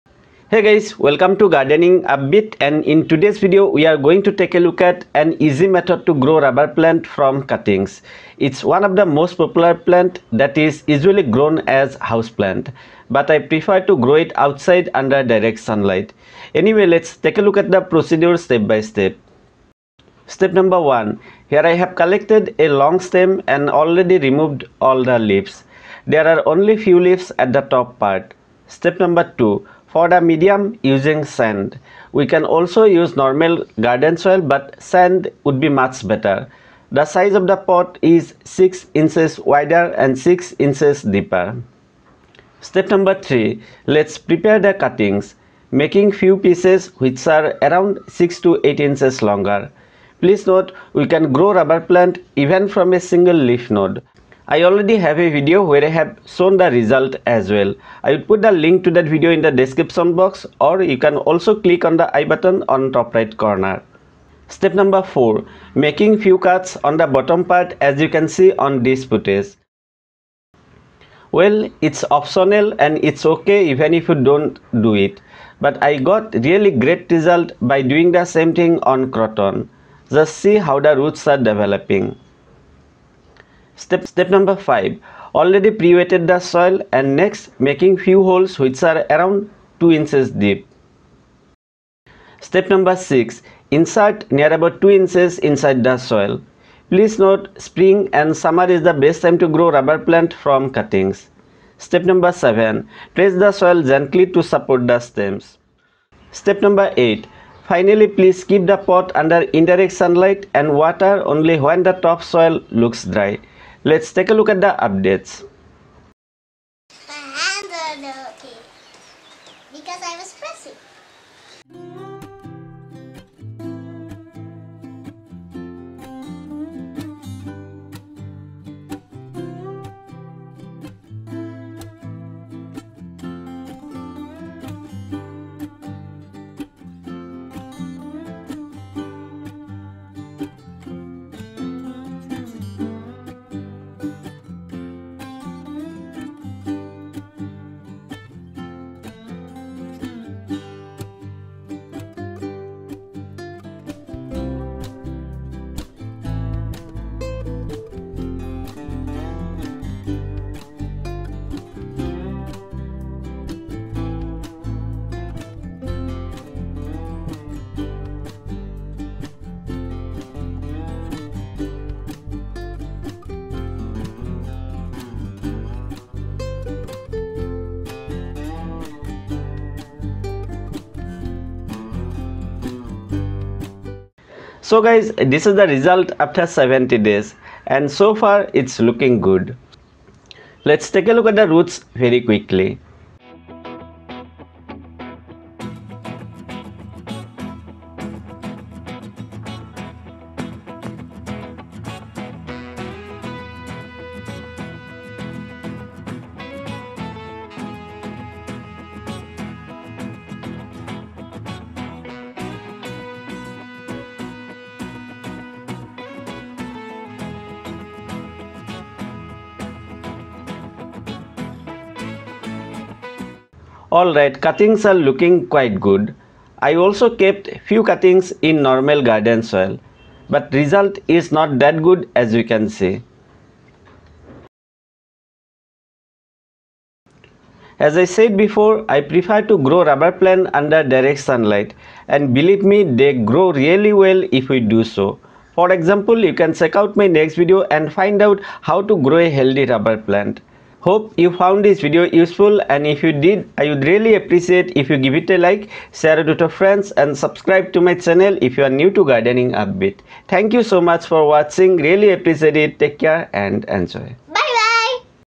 SUPER EASY Method To GROW Rubber Plant From Cutting!
How to grow Rubber plant from cutting? Rubber plant or Ficus Elastica is one of the most popular plants which is usually grown as houseplant or indoor plant. Here in this video, I have shared an very easy method of propagating Rubber plant. By following this method, you can grow as many plants as you like.

Links To Mentioned Videos -